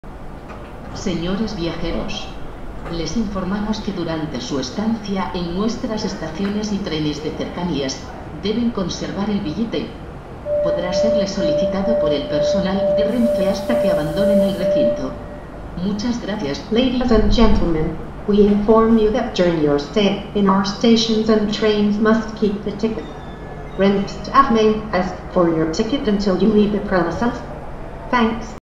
La Colina; general station announcement (20120203)
Automatisch omroepbericht opgenomen op het station La Colina (geleden aan de spoorlijn Málaga - Fuengirola (E)).

Het bericht betreft een melding, over het moeten hebben van een geldig vervoerbewijs op de stations en in de trein.

Opgenomen op 3 februari 2012.